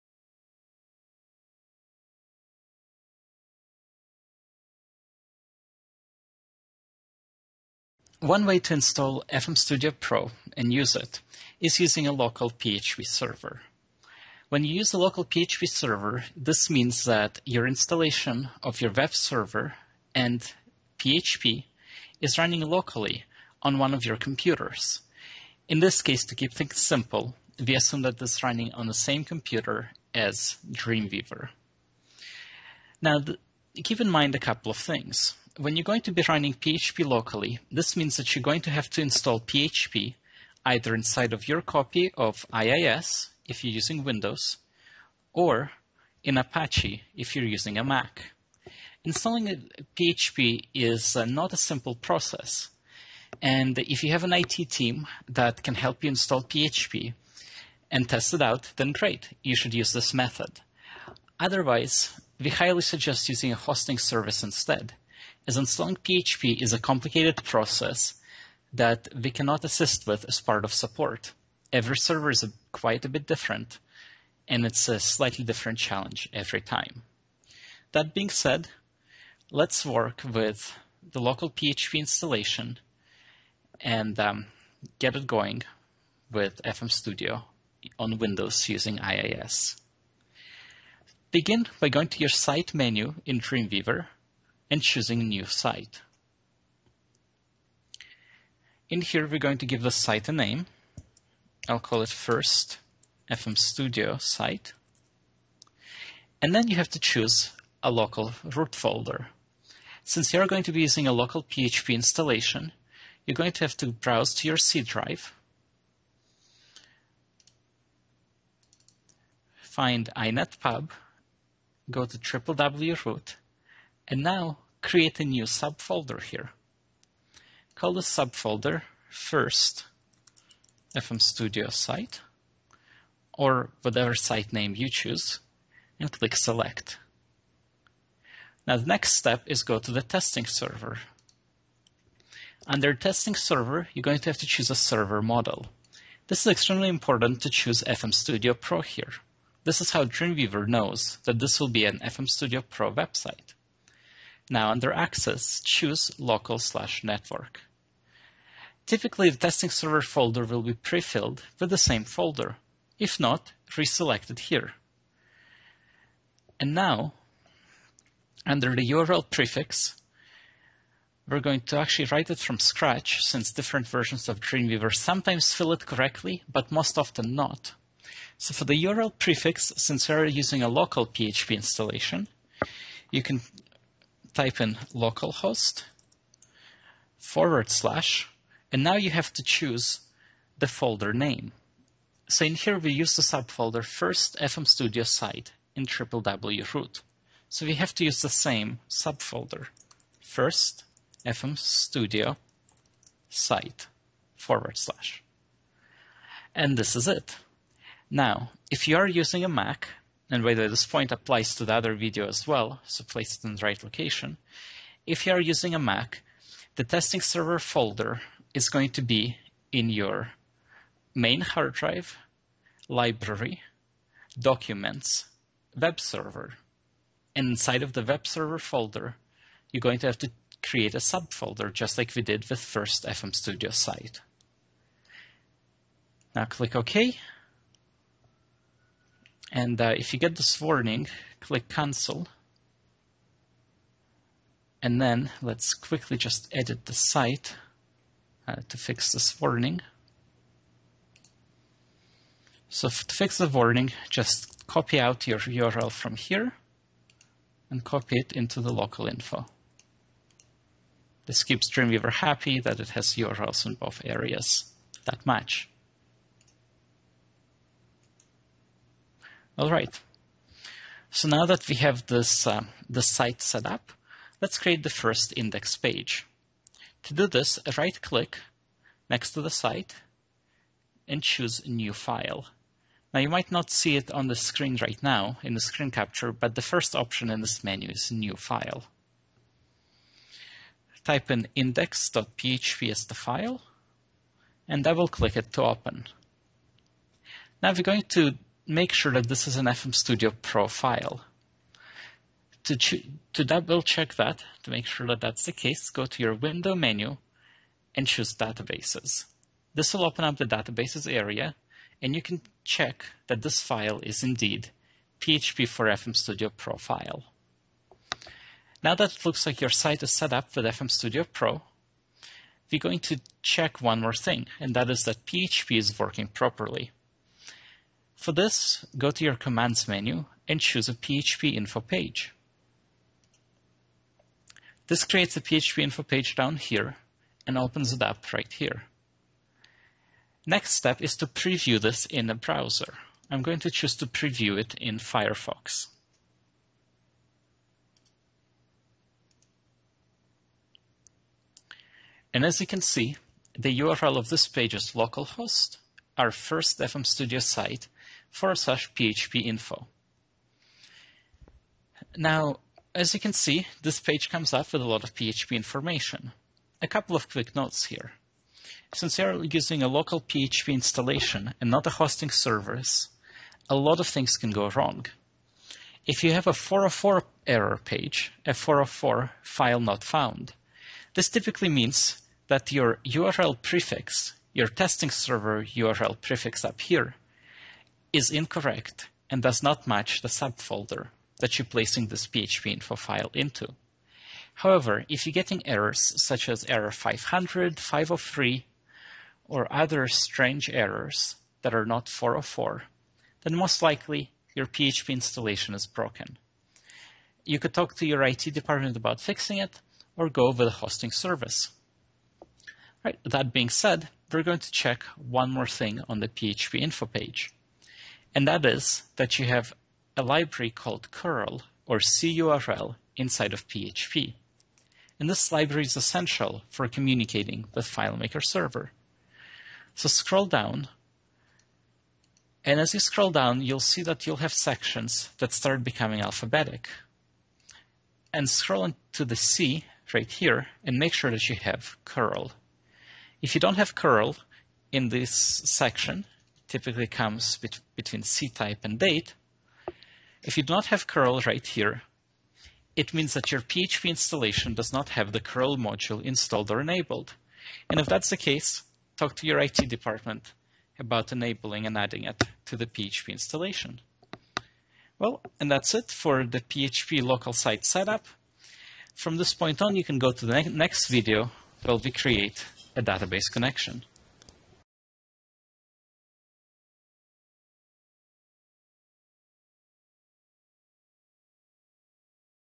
Installing PHP Locally for FMStudio Pro and Dreamweaver CS4
In this instructional FMStudio Pro you will learn how to set up PHP locally with FMStudio Pro.